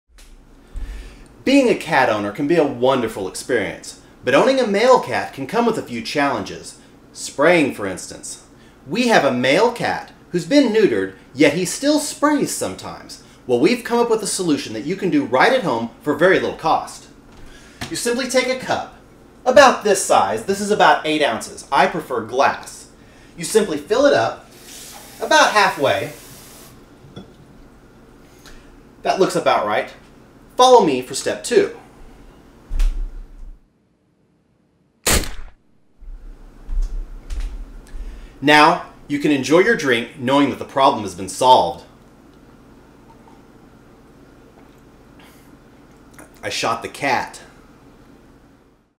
How to stop your cat from spraying
This video shows a home solution to stop a neutered male cat from spraying.  Often times when a male cat gets neutered, he will stop spraying, but not always.  This remedy is very successful in stoping the spraying problem.